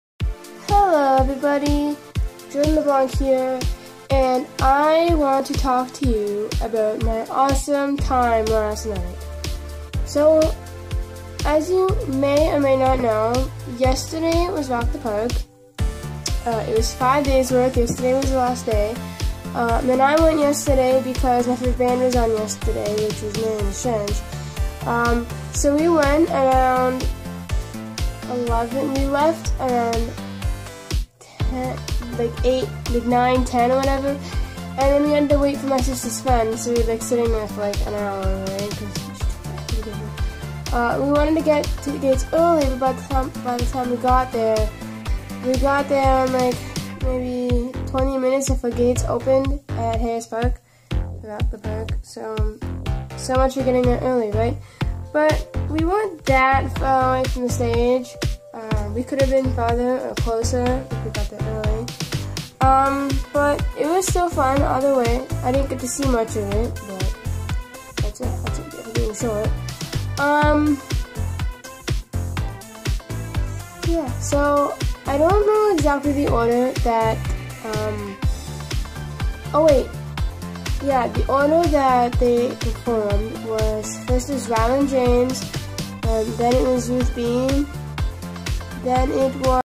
I touched josh ramsay on the chest!!!
I cant believe i was able to see marianas trench live and touch josh OMG AAAHHH